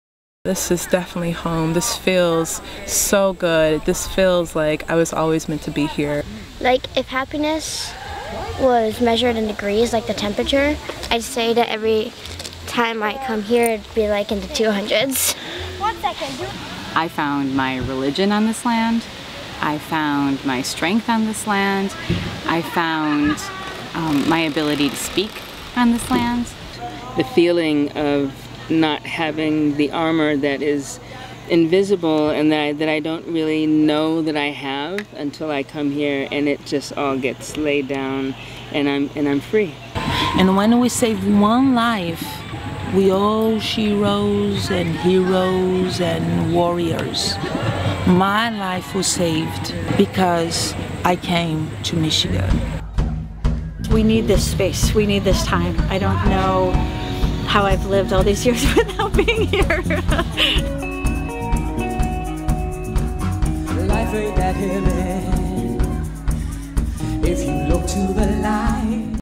Because I Came to Michigan - A video about the Michigan Womyn's Music Festival
"When we save one life, we are all sheroes and heroes and warriors. My life was saved because I came to Michigan."

featuring the music of Hanifah Walidah